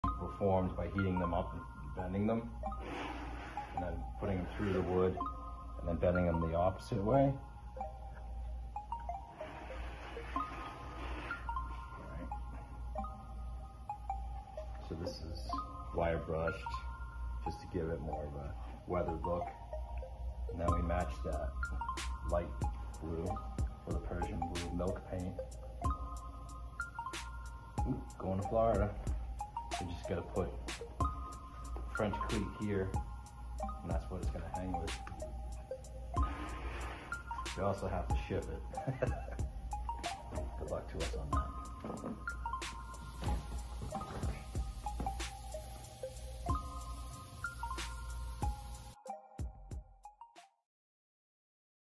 Mounting a Ceramic Bowl
We were asked to mount a ceramic bowl. We used custom formed Plexi to clip onto the edge of the bowl through the back of the wooden frame.